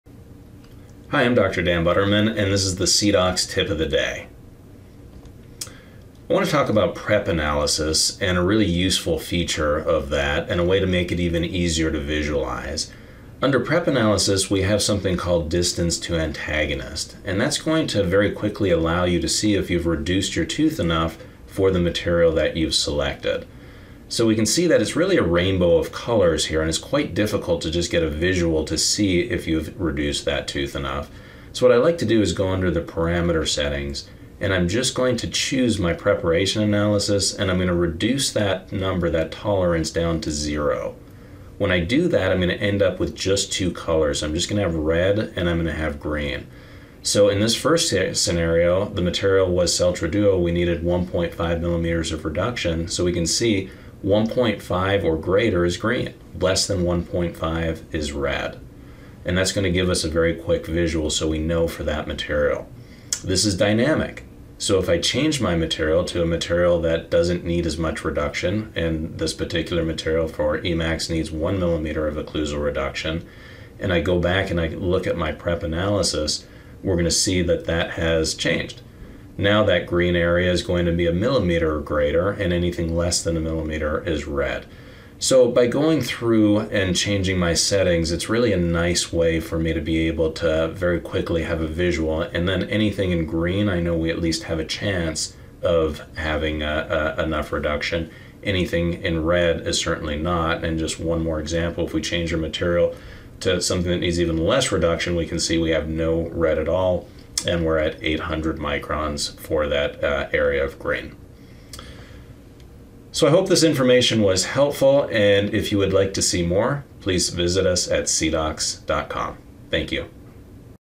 Tip of the Day: Prep Analysis
This video will discuss the prep analysis feature in the CEREC software.